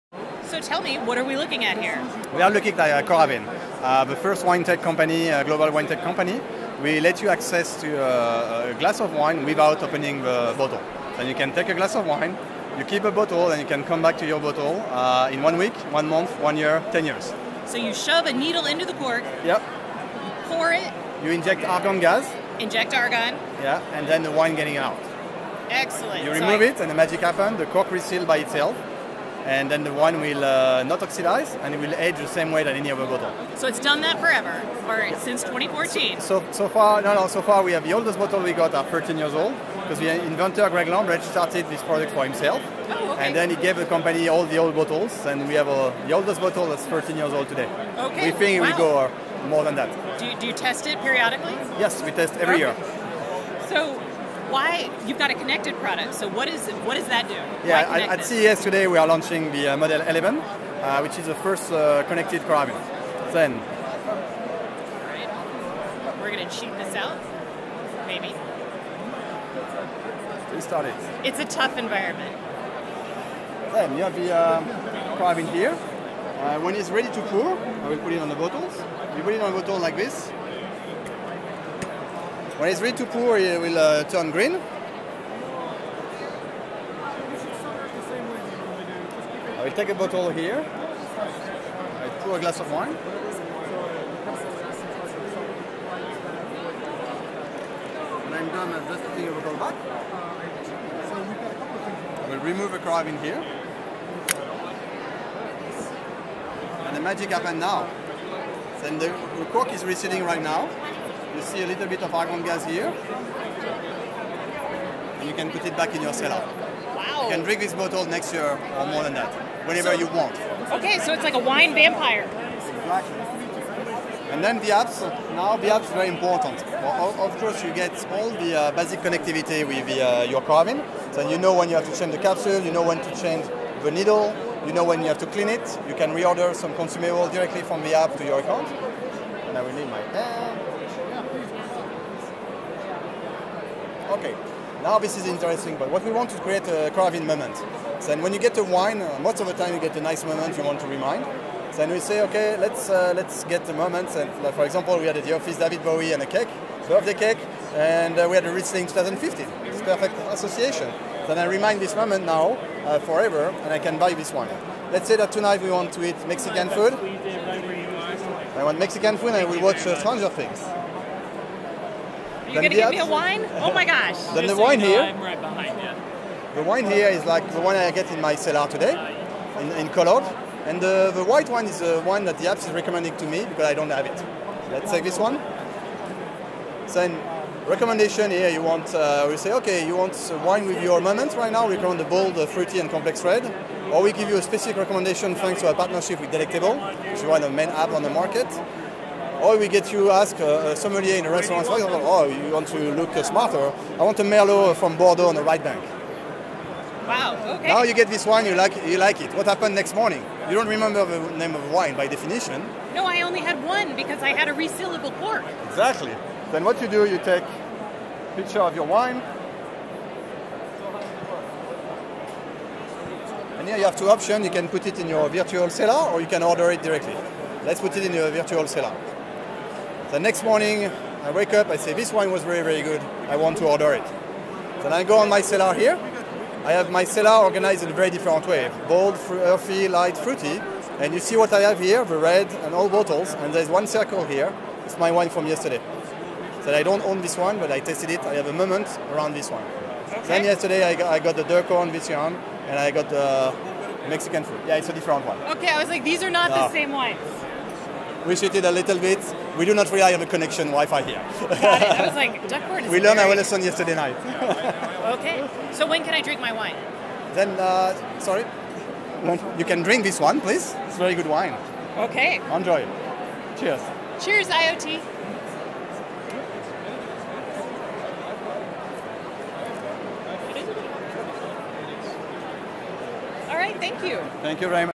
Meet Coravin, a smart device for wine
At CES 2018, Coravin was showing off their newest product. Aside from keeping wine fresh by piercing the cork for pouring and then injecting argon gas, this device has a connected app to track data and your favorite wines.